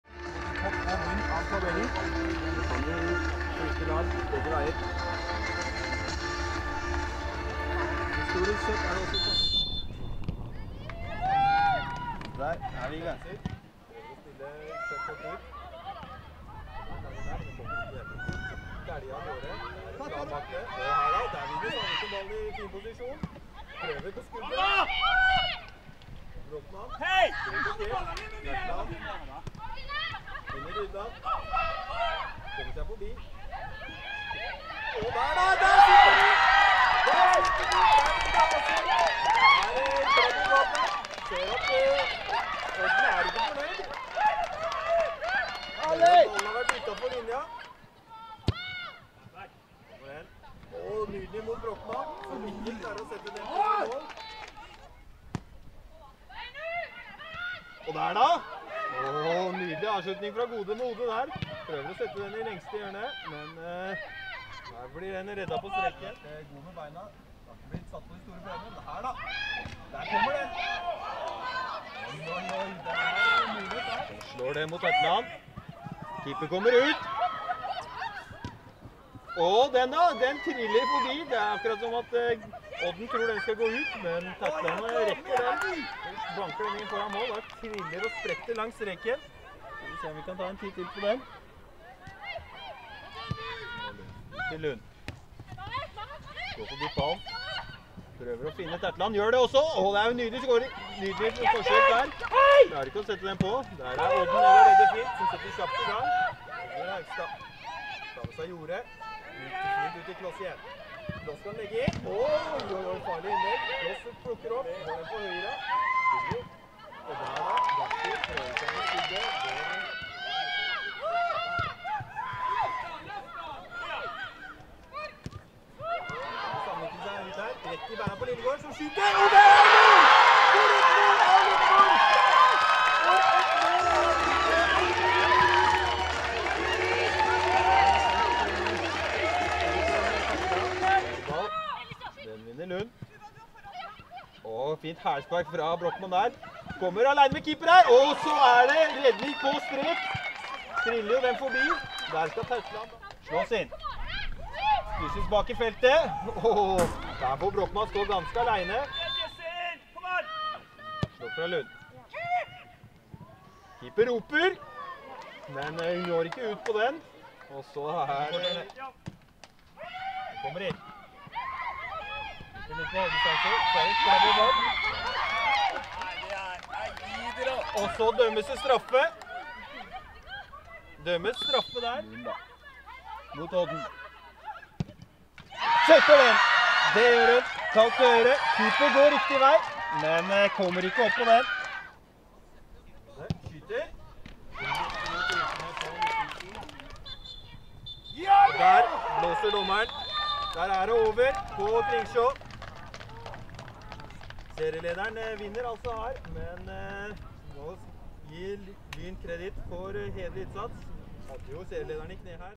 Lyn – Sandviken 1-2
10. runde i Toppserien 2021.

Mål: Vilde Hasund (10'), Runa Lillegård (71'), Marit Bratberg Lund (straffe, 91')